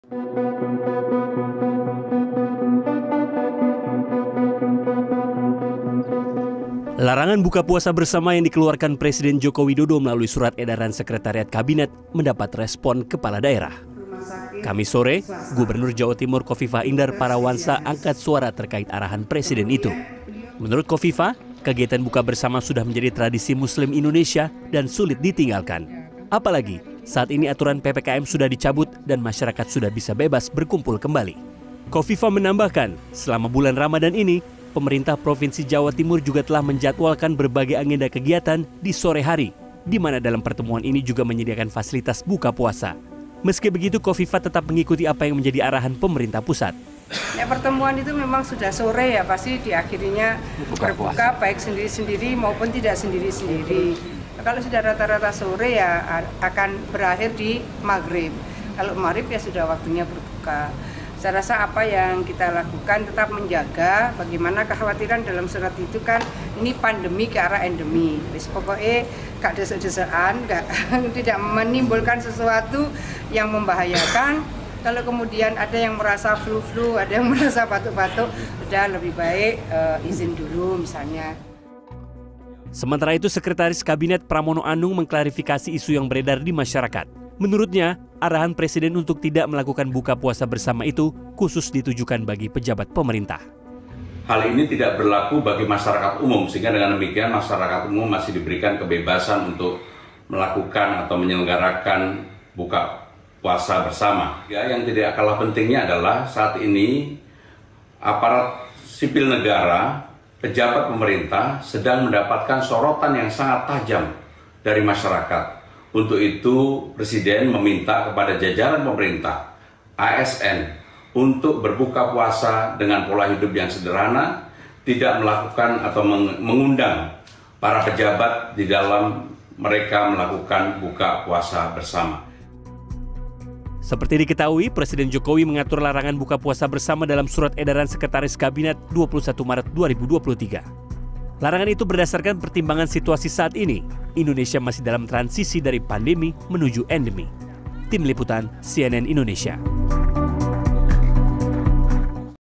Polemik Presiden Melarang Pejabat Buka Puasa Bersama
Terkait larangan berbuka puasa bersama. Pihak istana memberikan klarifikasi, jika arahan Presiden Joko Widodo itu, khusus ditujukan kepada pejabat pemerintah, dan bukan untuk masyarakat.

Sementara itu, Gubernur Jawa Timur, Khofifah Indarparawansa menyatakan, larangan buka puasa bersama ini sulit dilakukan karena sudah menjadi tradisi. Apalagi, agenda pertemuan dan kegiatan Pemprov Jatim di bulan puasa, sudah terjadwal di sore hari.